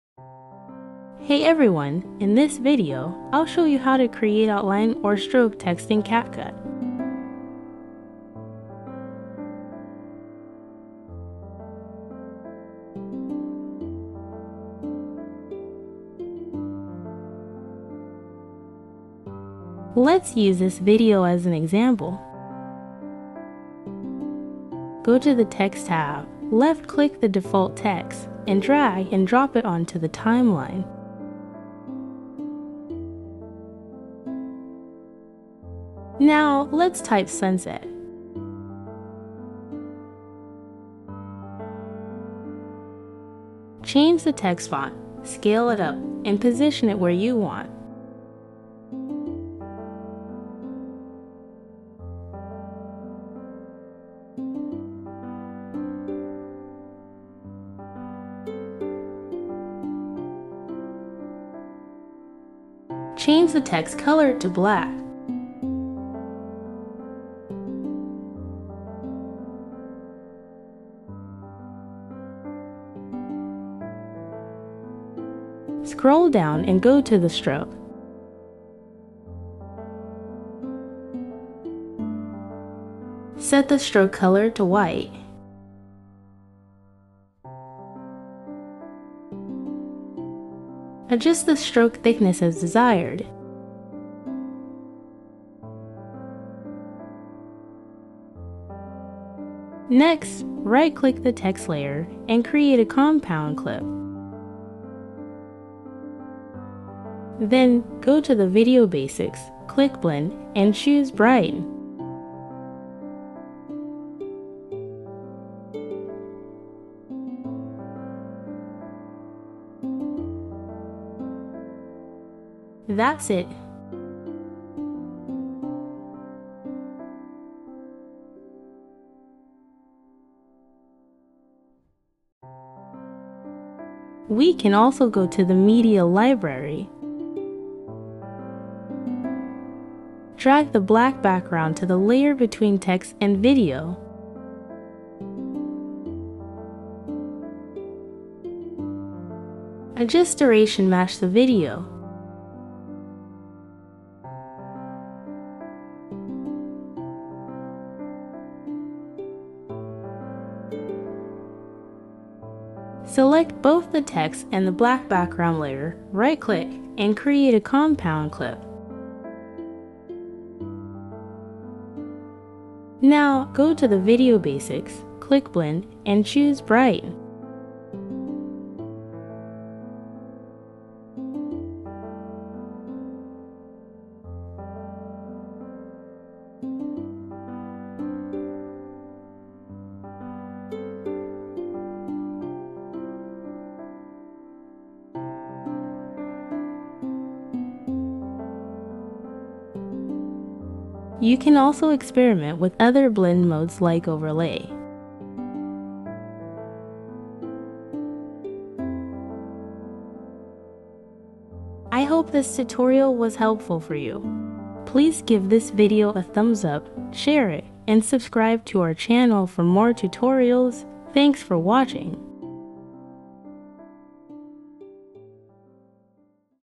CapCut PC Tutorial: How to Create Text Outline or Stroke Text Effects
Learn how to add stunning text outline or stroke effects to your videos using CapCut on your PC!